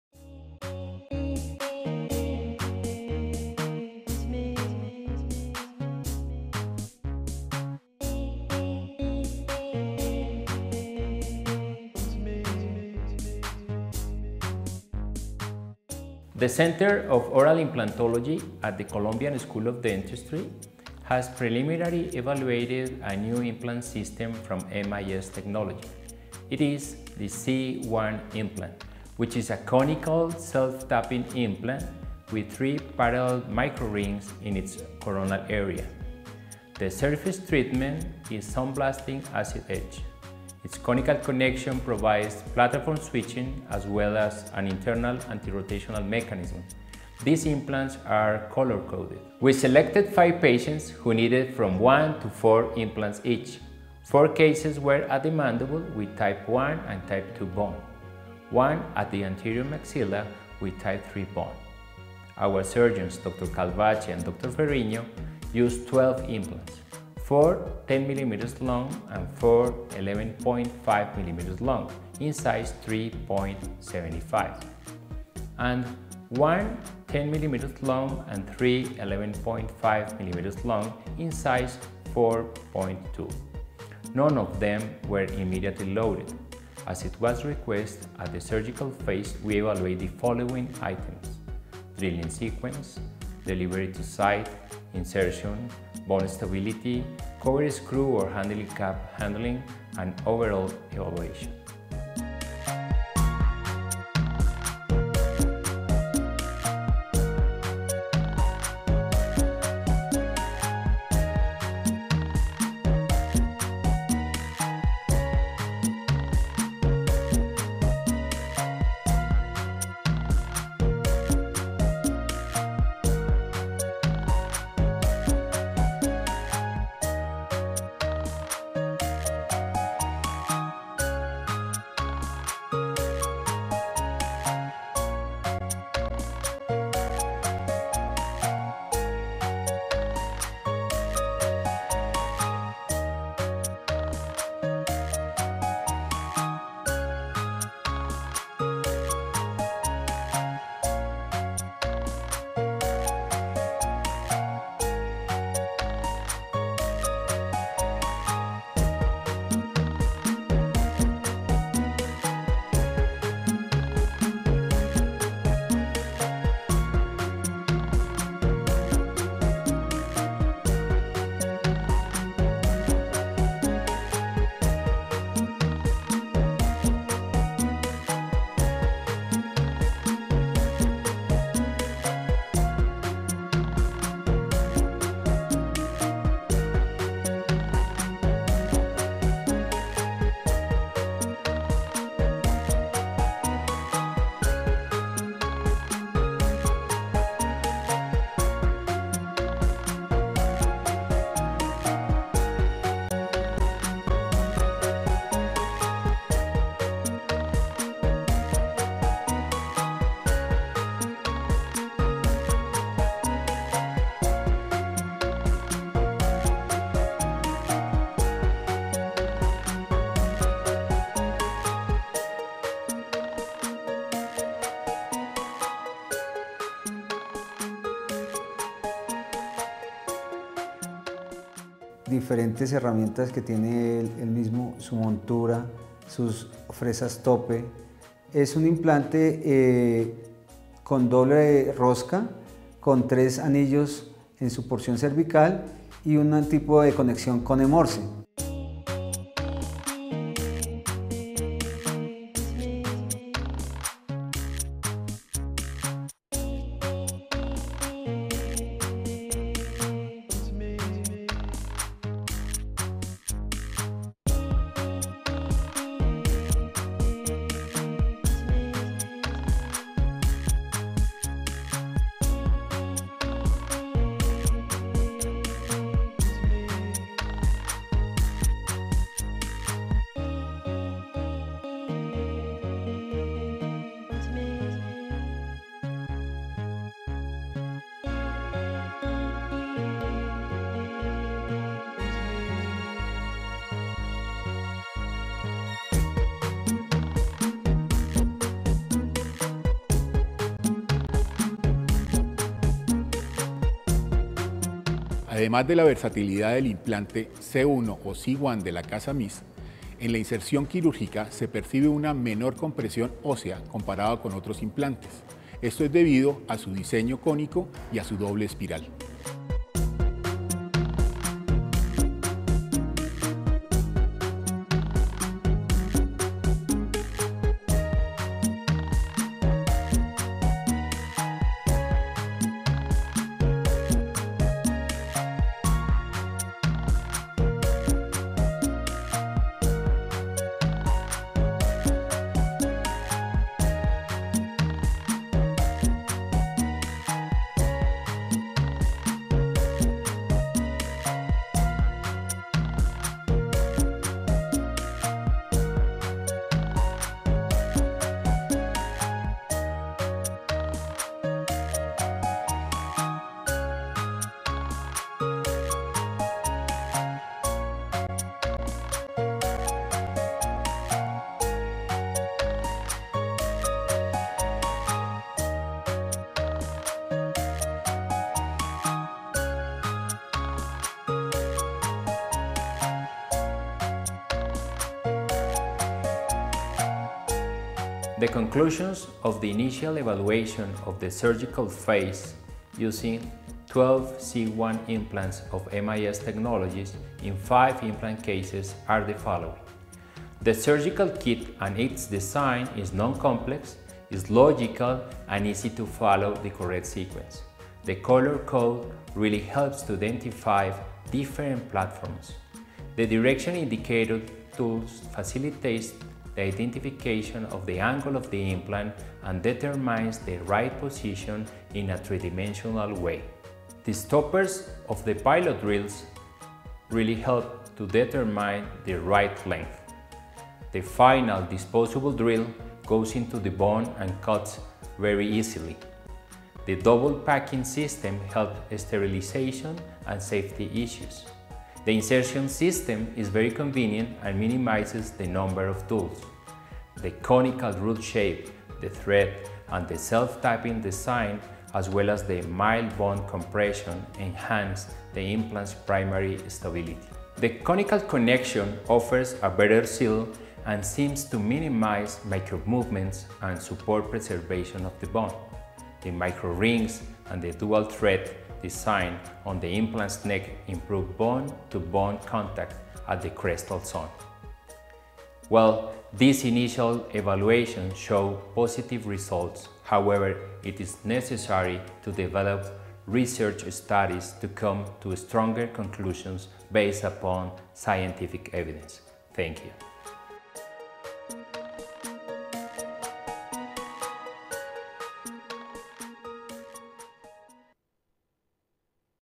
C1 - Immediate Loading Case Study
A case study of 5 patients, who needed 1-4 implants each with C1 Conical Connection Implants, Colombian School of Dentistry.